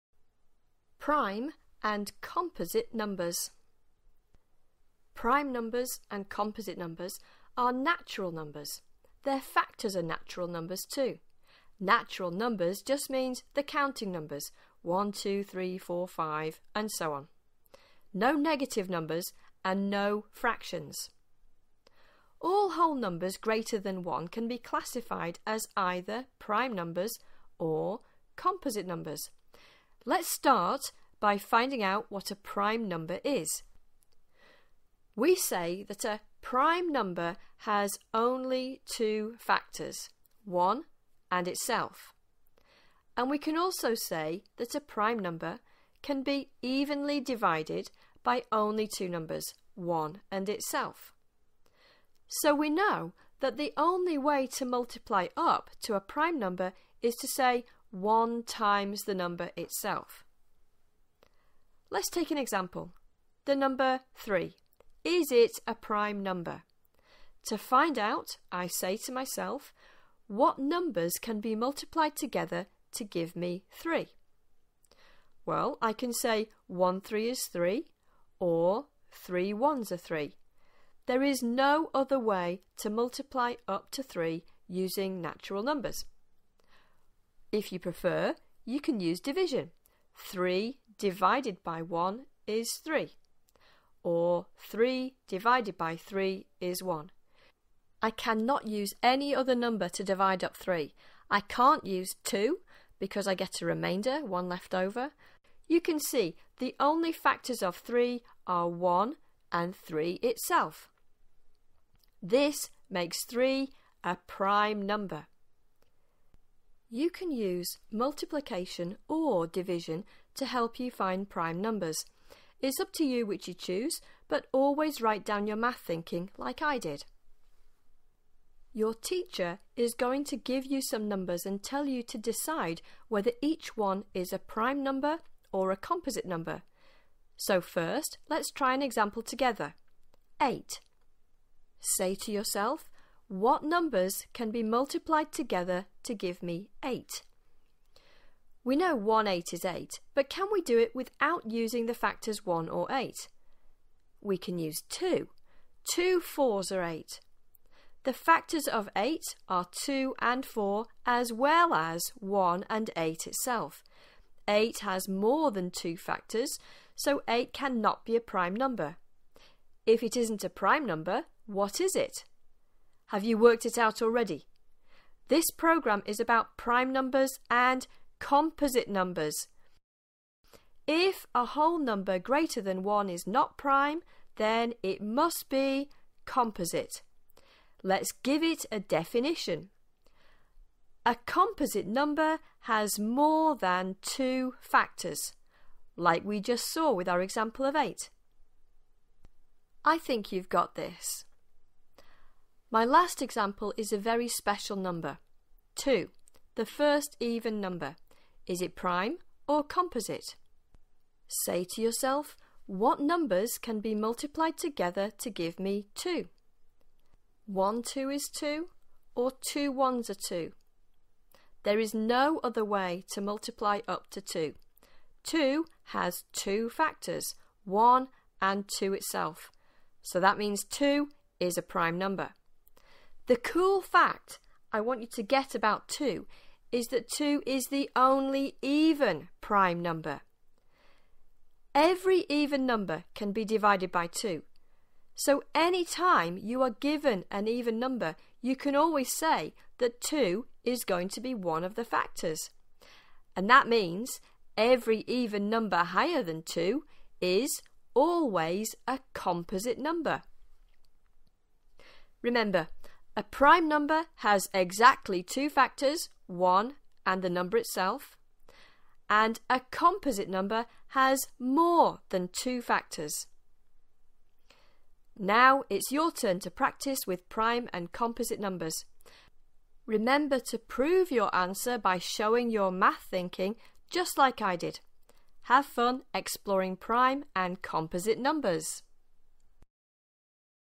Prime and Composite Numbers
If this helps you, please click LIKE and help me too. Thanks!

Easy intro to prime and composite so that it MAKES SENSE.
Defined through division AND multiplication, though focus is on multiplication.
1. Prime is shown through the example of 3
2. Second example given is 8. Not prime, so composite.
Definition of composite number: more than 2 factors
3. Third example is 2, the only even prime number.